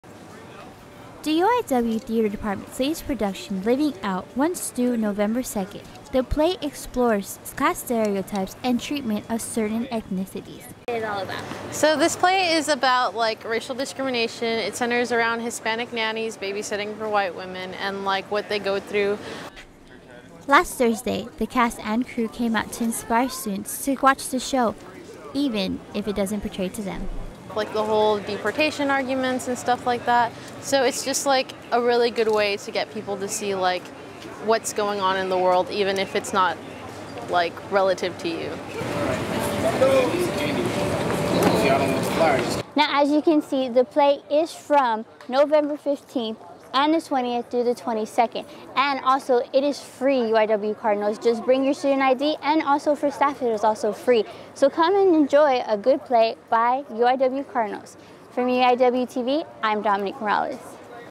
Living Out (UIW Theater Department)
The latest theater play performed by some of UIW's finest. To learn more about the production, we heard over to reporter, Dominique Morales.

Directors:
Hannah Wadlow (Program)
Scott Wild (News)
Faelany "Nani" Melendez-Lopez (Entertainment, Spanish)
Maritza Garcia (Communications, Spanish)
Alejandro Jimenez (Operations)